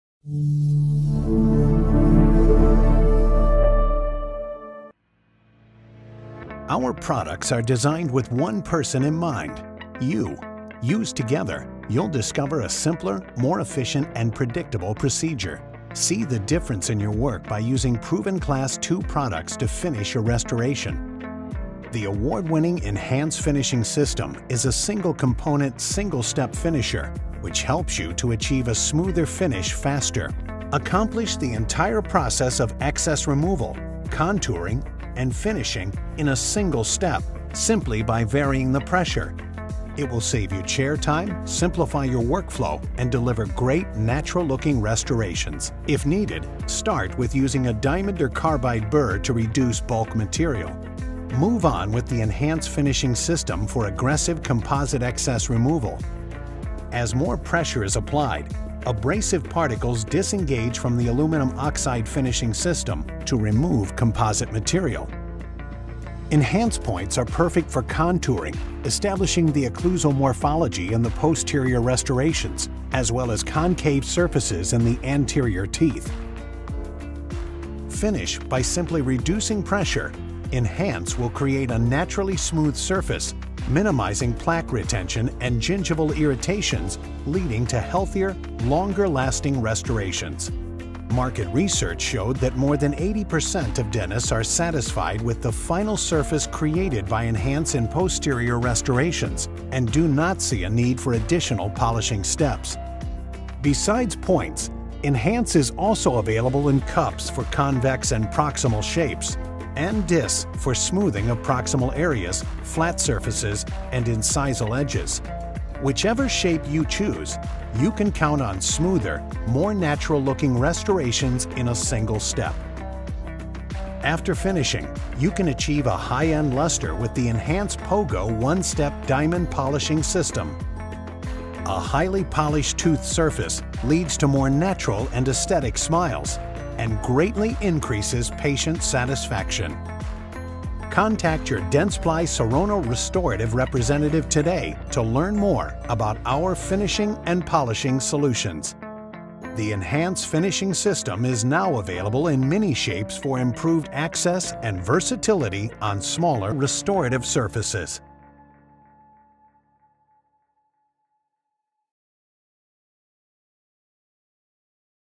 Enhance Demo Video
Accomplish the entire process of excess removal, contouring and finishing in a single step simply by varying the pressure. It will save you chair time, simplify your workflow and deliver great natural looking restorations.